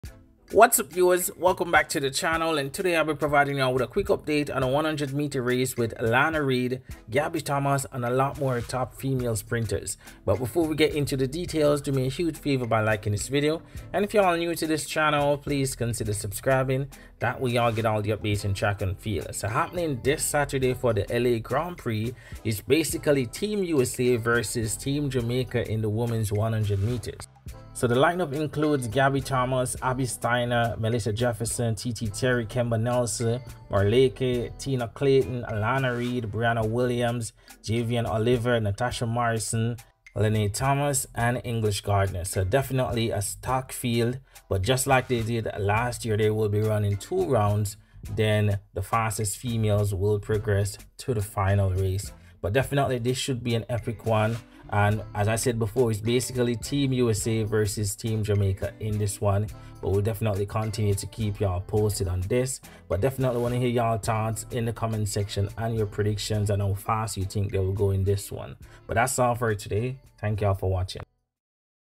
Wow! Alana Reid Battles Gabby Thomas In Epic 100m At LA Grand Prix 2024
Alana Reid 100m 2024
Gabby thomas 100m 2024